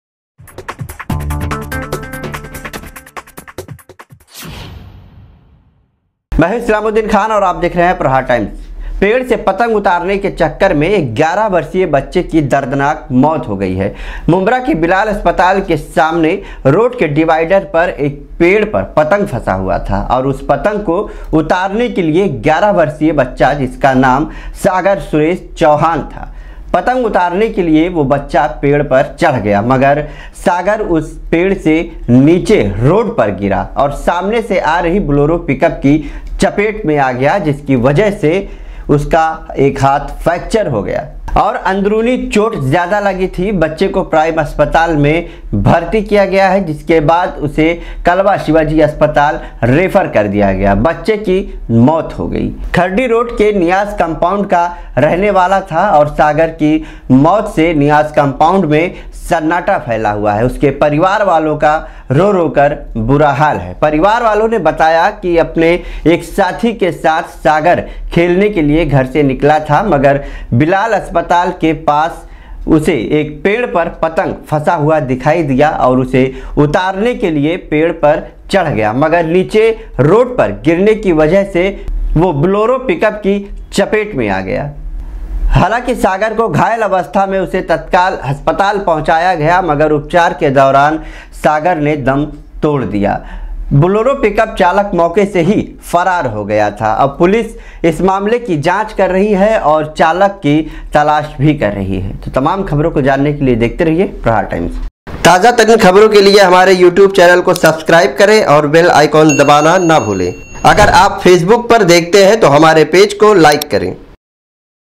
Mumbra पतंग के चक्कर मे बच्चे की दर्दनाक मौत || Islamuddin Khan||Aapka Prahar Times
Mumbra पतंग के चक्कर मे बच्चे की दर्दनाक मौत
पतंग उतारने के दौरान हुआ हादसा
पेड़ से गिरा बोलेरो  पिकअप की चपेट में आगया
इलाज के दौरान बच्चे की हुई मौत